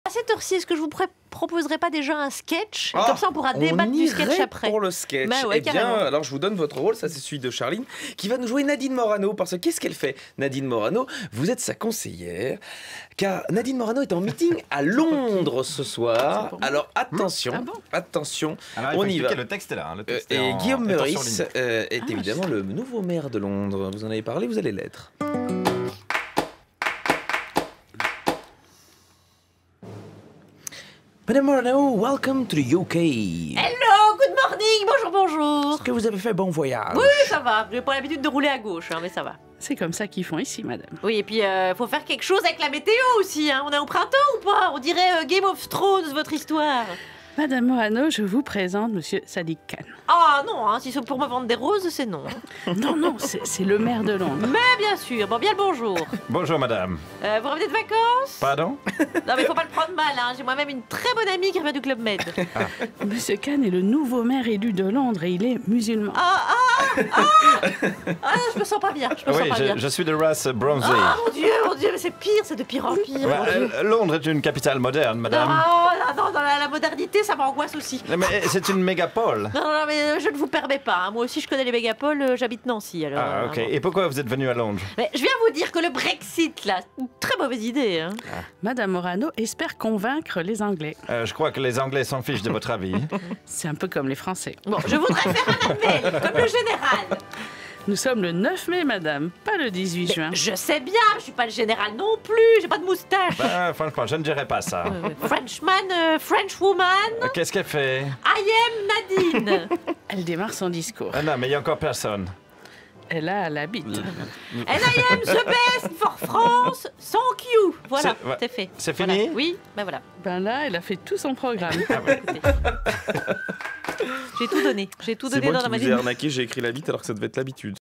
Je suis de race bronzée - Le sketch avec Sandra Laugier
Sandra Laugier est la conseillère de Nadine Morano, en meeting à Londres. Elle rencontre Sadiq Khan, le nouveau maire de la capitale britannique.
Avec Charline Vanhoenacker, Sandra Laugier, Guillaume Meurice et Alex Vizorek.